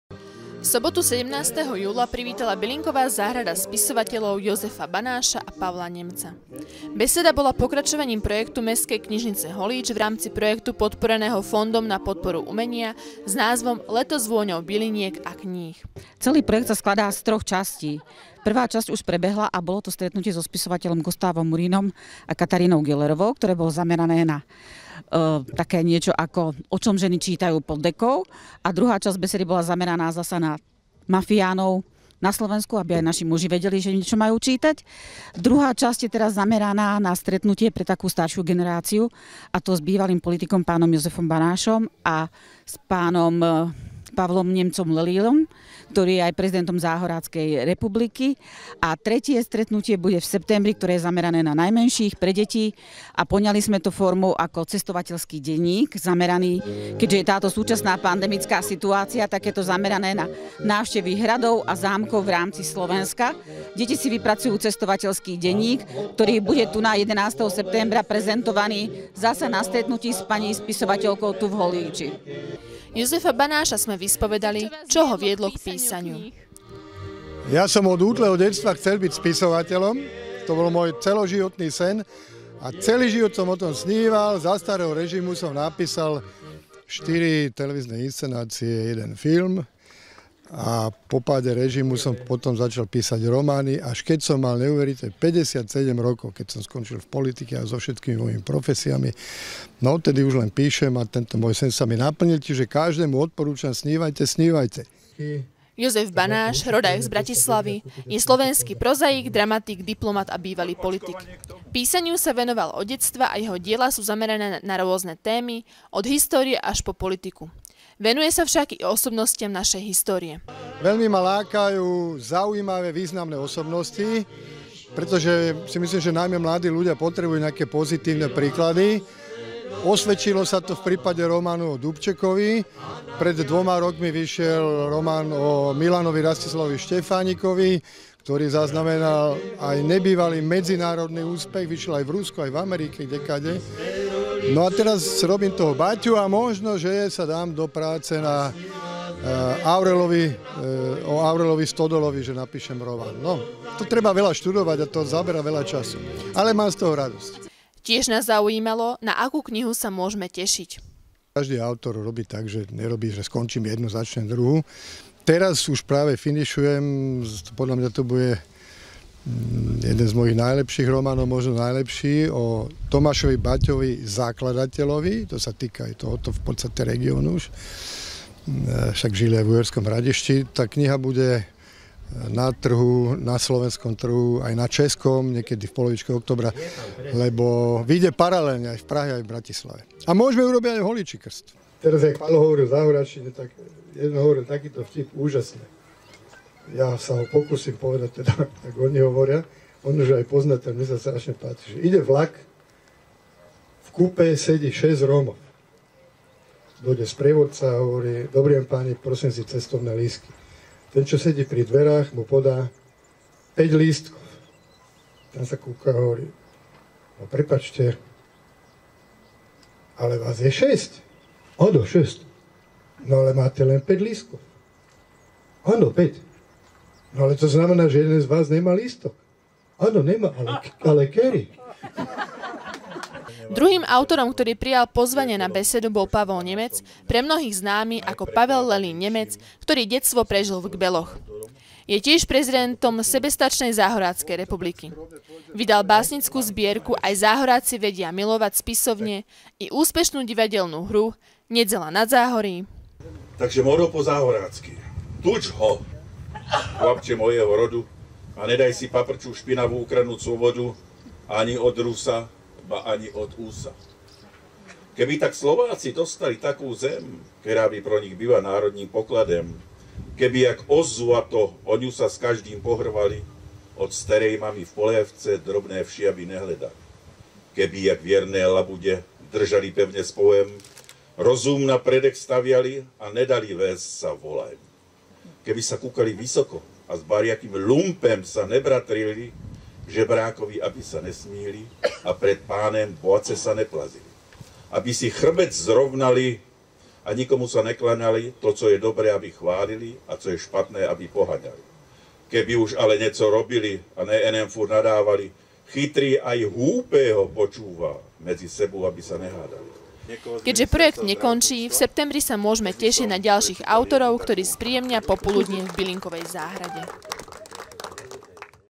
V Bylinkovej záhrade so spisovateľmi
Hosťami druhej besedy v Bylinkovej záhrade boli známi spisovatelia Jozef Banáš a Pavol Nemec Lelín. Zatiaľ čo prvý autor sa venuje najmä písaniu historických a politicky zameraných diel, pre Pavla Nemca je hlavnou témou jeho rodné Záhorie. O hudobné spestrenie sa postarali Záhorácki heligónkari.